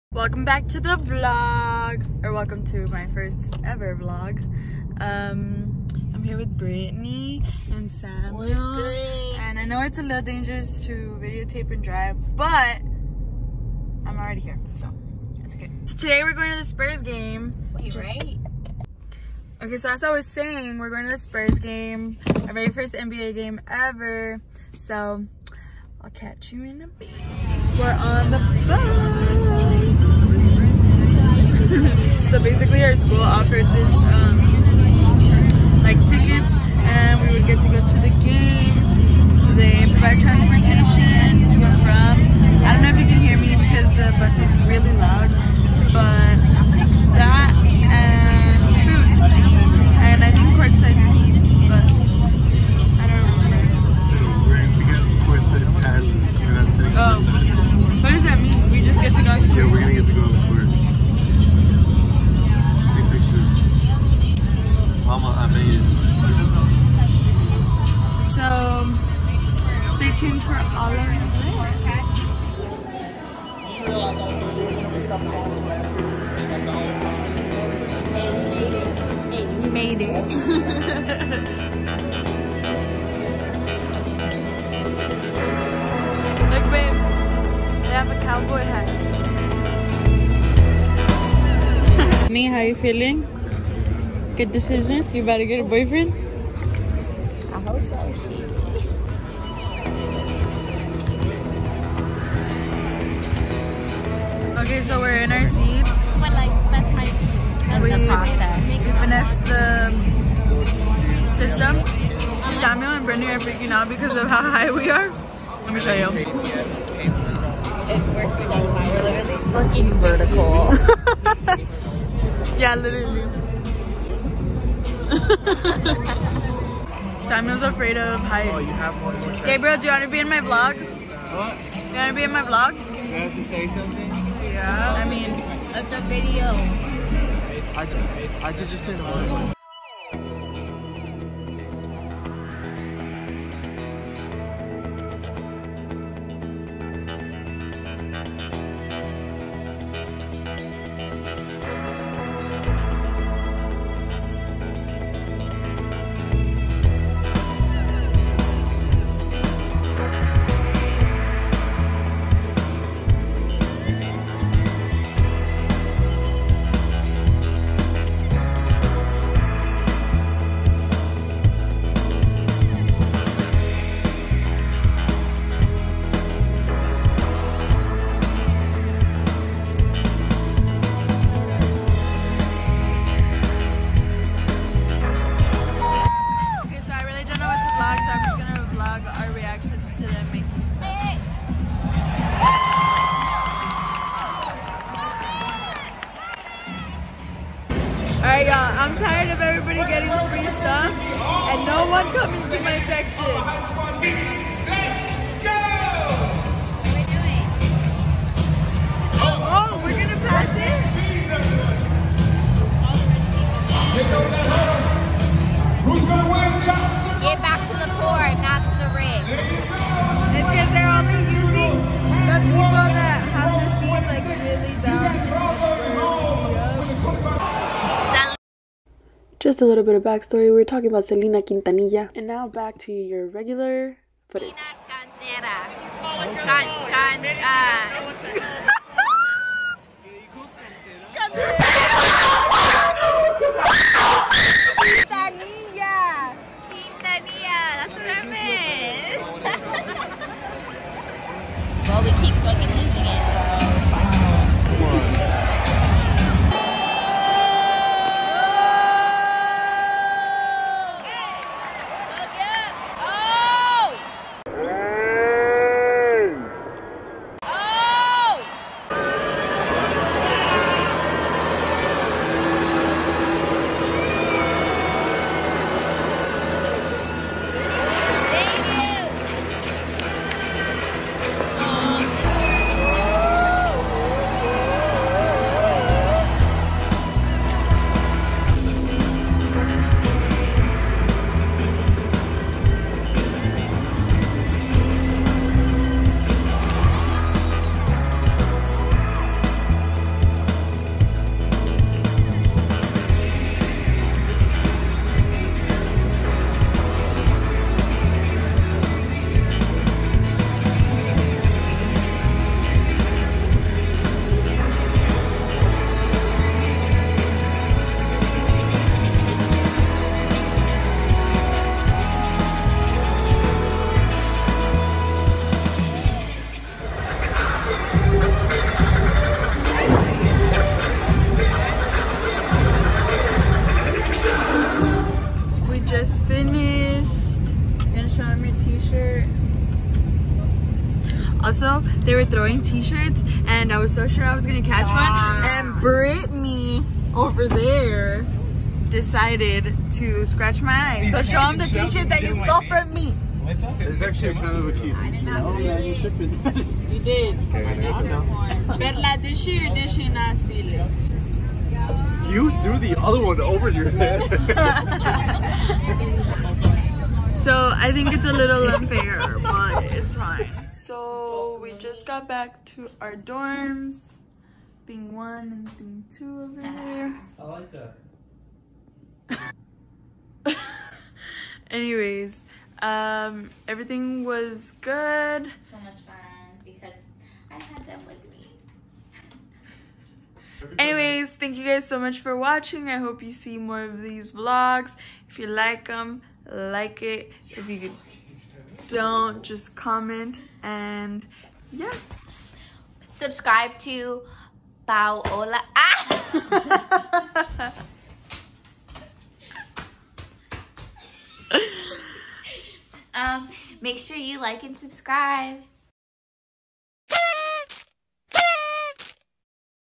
Going to a Spurs Game w/ friends vlog!
Welcome back guys! This is my first vlog ever on my channel and I hope that you guys enjoy! We had a lot of fun but not a lot of footage so I will get better at making these soon, promise!!